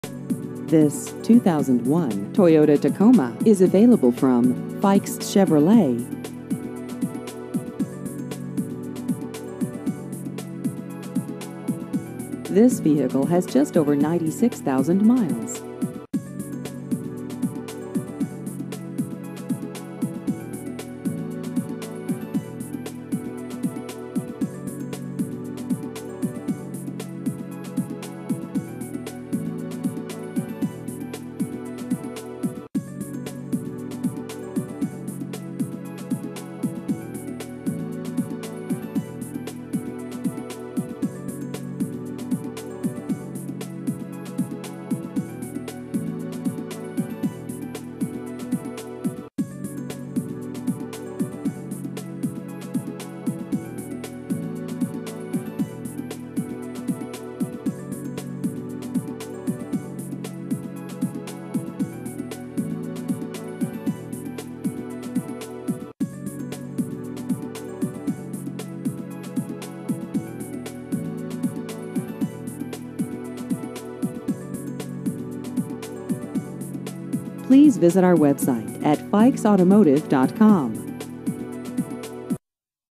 2001 Toyota Tacoma Used Cars Hamilton AL
This 2001 Toyota Tacoma is available from Fikes Chevrolet.